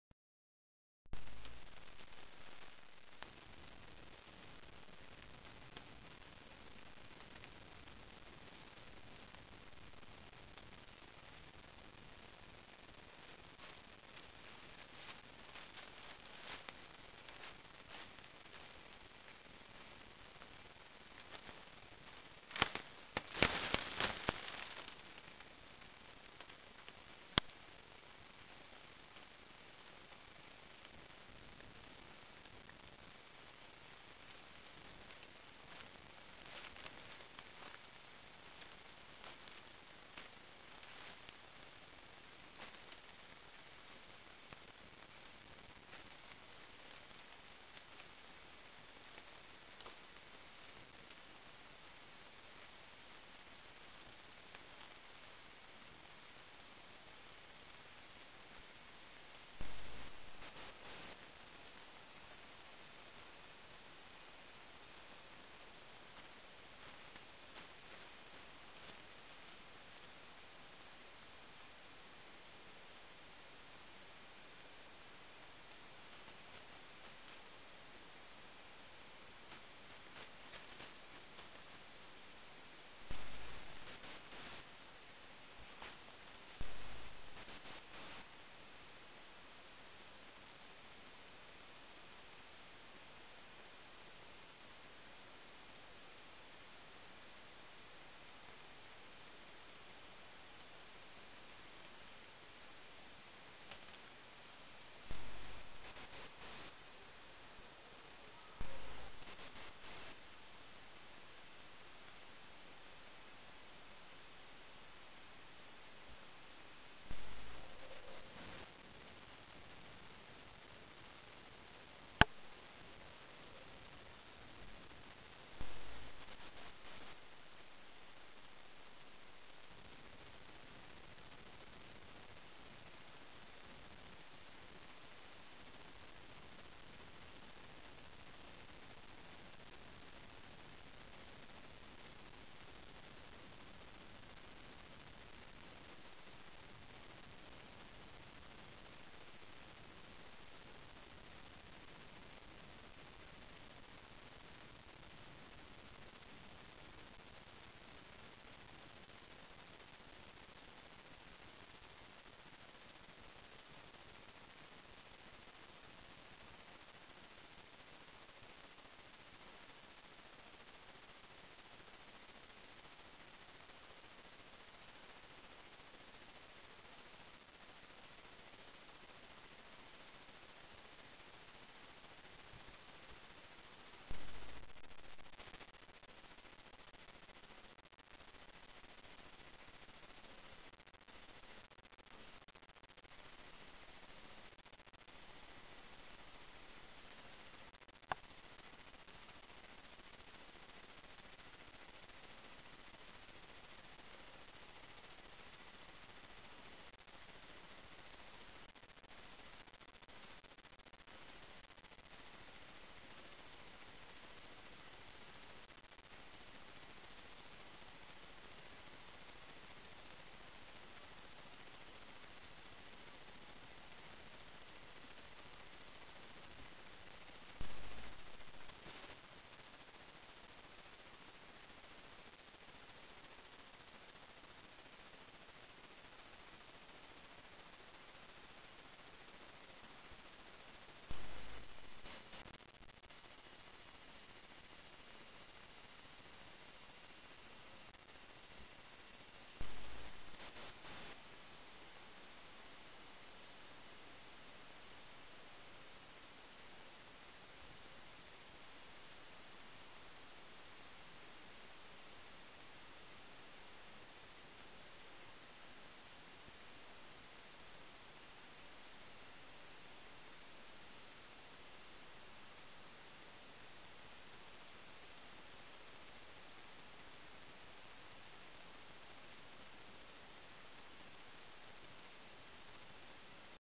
Wildlife 63 (Scoutguard SG 520) - Photo traps
Zábery divej zveri
Na záberoch môžete vidieť:
Jeleň lesný (Cervus elaphus)

Srna lesná (Capreolus capreolus)
Líška hrdzavá (Vulpes vulpes)

Jazvec lesný (Meles meles)

Ak si fanúšikom fotopascí a máš aj nejaký záber, s ktorým by si sa podelil neváhaj a pridaj sa

Images of wild animals
You can see:
Deer  (Cervus elaphus)
Roe Deer  (Capreolus capreolus)
Wild boar (Sus scrofa)
Red fox (Vulpes vulpes)
Forest badger (Meles meles)